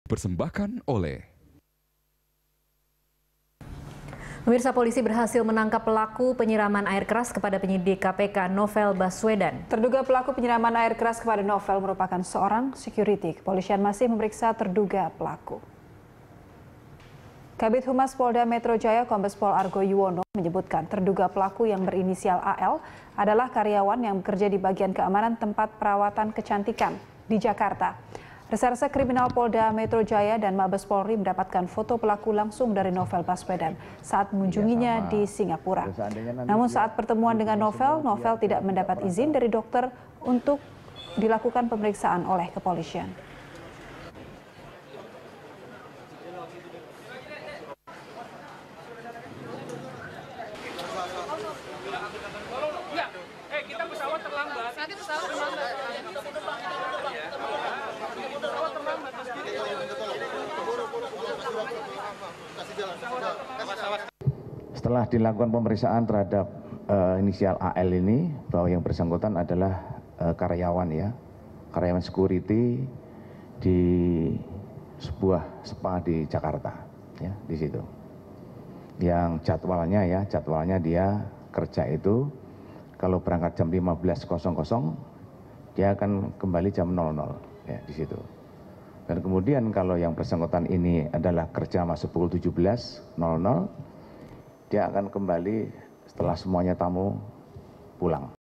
Penyerang Novel Berinisial AL Berprofesi Sekuriti
Polisi menangkap pelaku penyiraman air keras kepada penyidik KPK Novel Baswedan, Kamis (11/5/2017). Pelaku merupakan seorang petugas sekuriti.

Facebook.com/BeritaSatuTV
Youtube.com/BeritaSatu